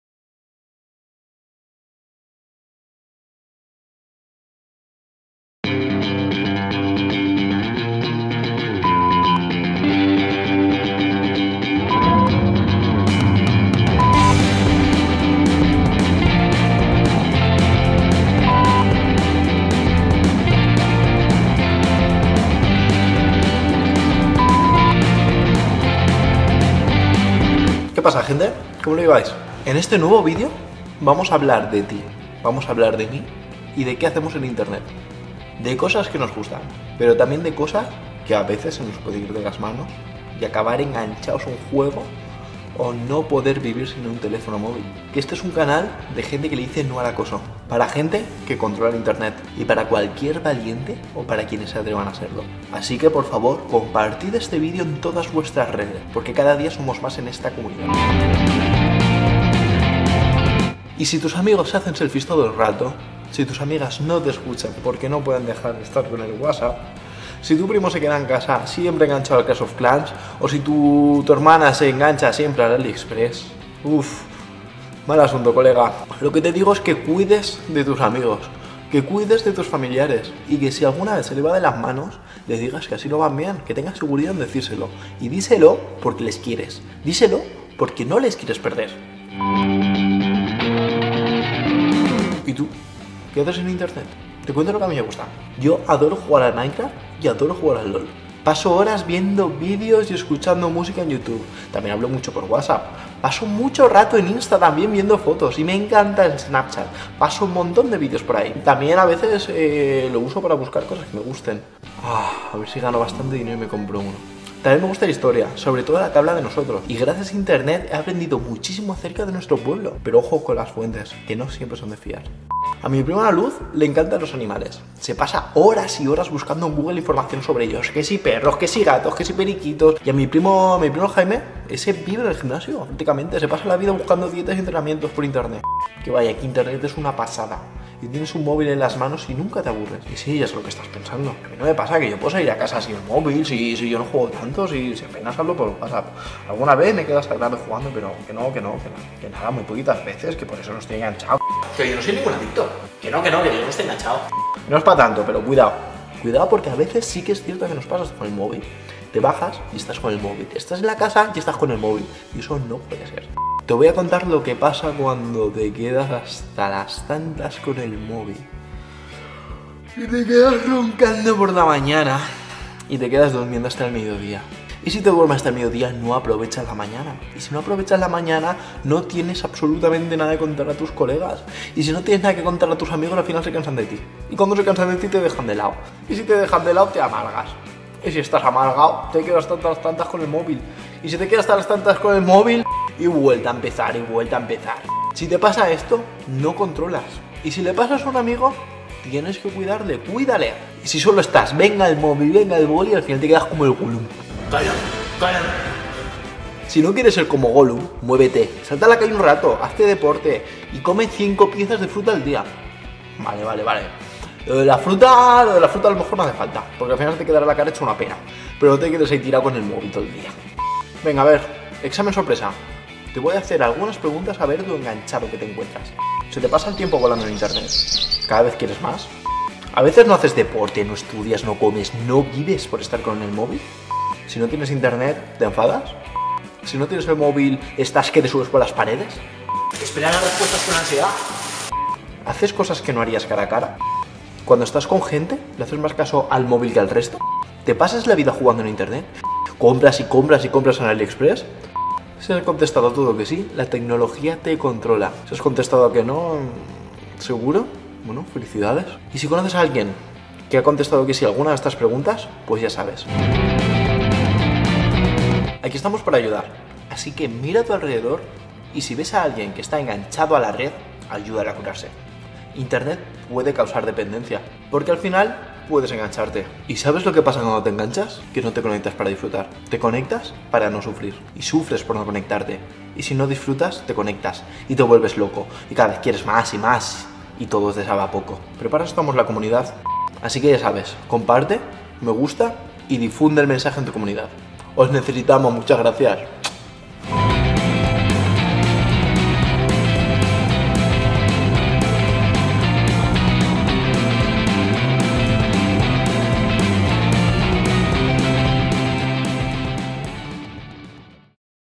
la vida online
Una acción de sensibilización de la Fundación Secretariado Gitano de prevención del ciberacoso y del uso problemático de Internet.